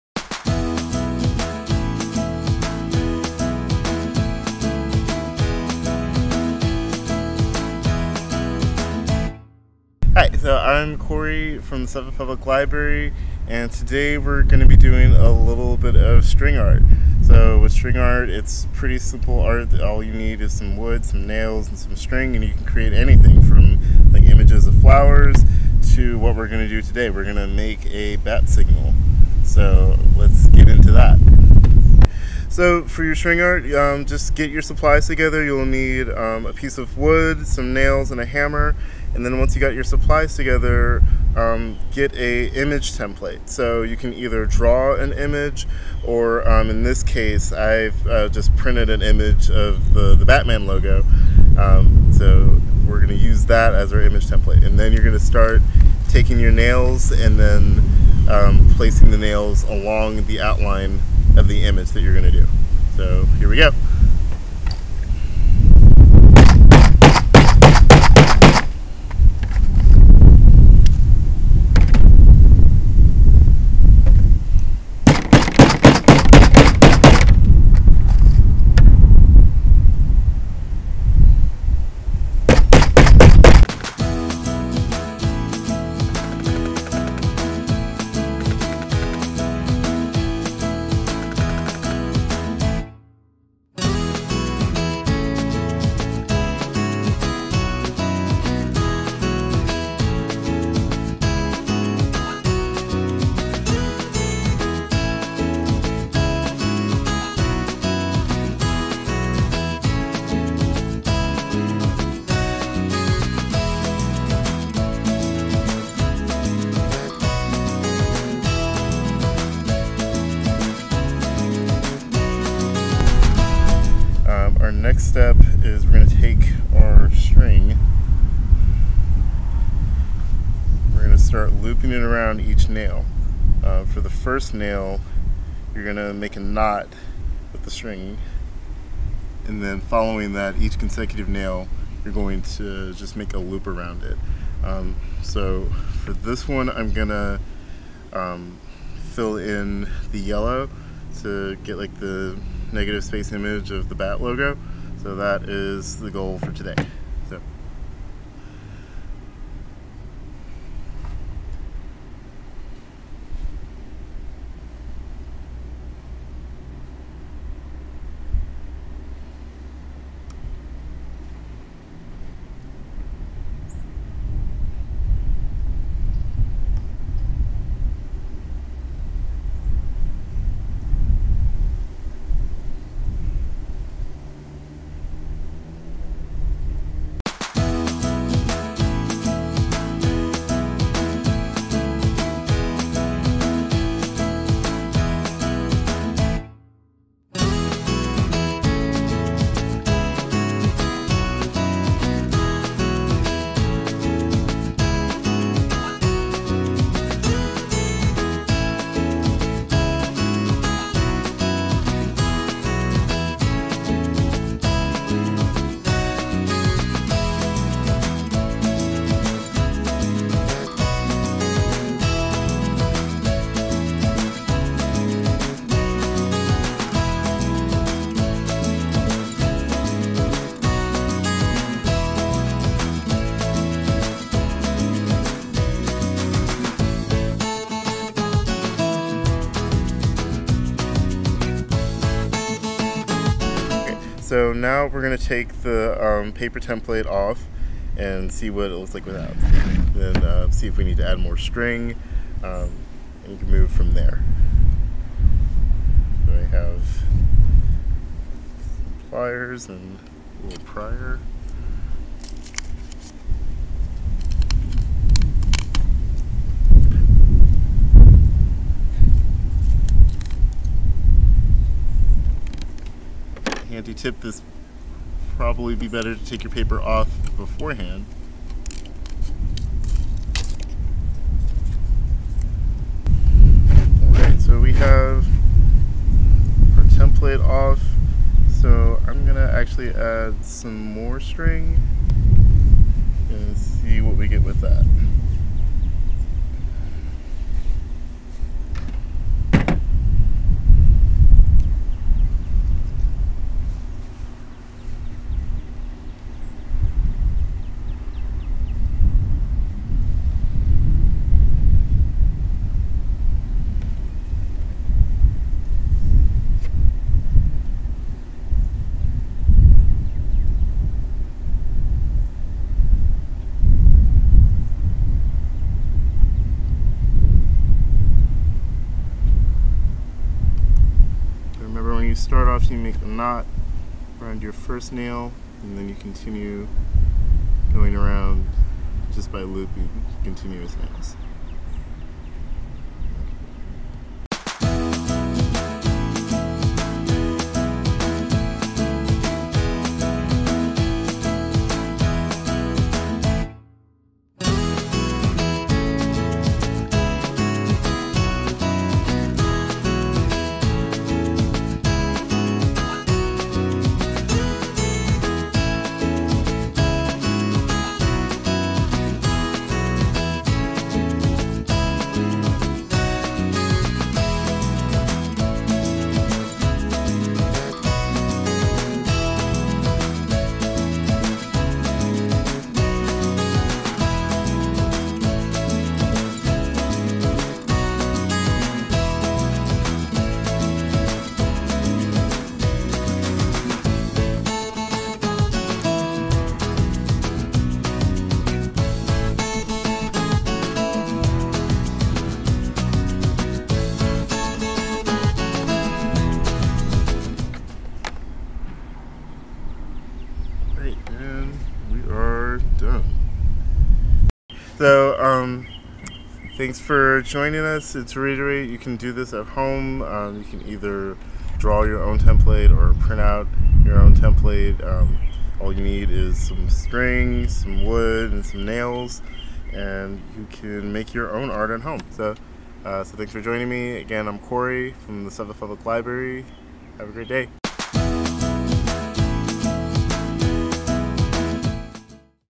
DIY with SPL: String Art
Holy string art, Batman! Cory walks you through how to make your own string art using a few basic supplies: wood, nails, string and a hammer!